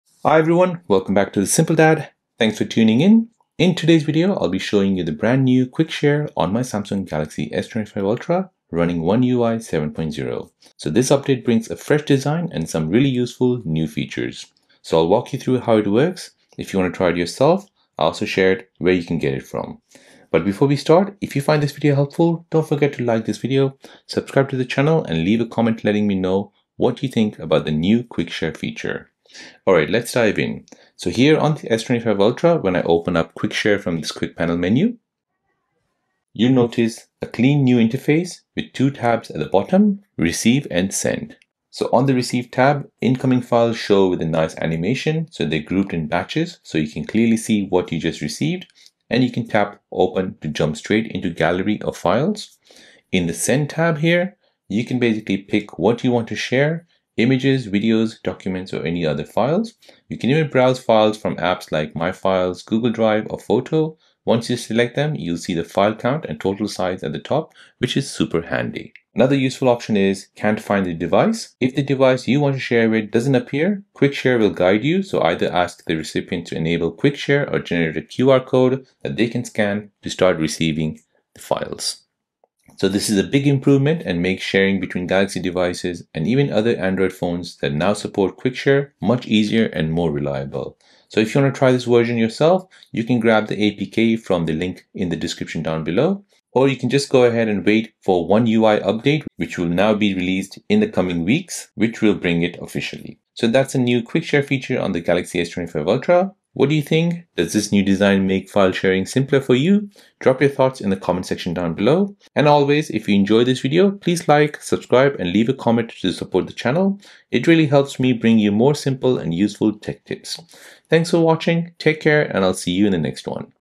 You NEED to Try the NEW Quick Share on Galaxy S25 Ultra!
🚀 The NEW Samsung Quick Share is here on the Galaxy S25 Ultra!
In this video, I’ll walk you through the latest Quick Share update on One UI 7, showing you the brand-new interface, Send/Receive tabs, QR code sharing, and all the new features that make file sharing faster and easier than ever.

📱 Works on:

Galaxy S25 Ultra (One UI 7)

Other Galaxy devices with One UI 7 / 6

Coming officially with One UI 8

👉 If you found this video helpful, don’t forget to:
📩 Comment your thoughts about the new Quick Share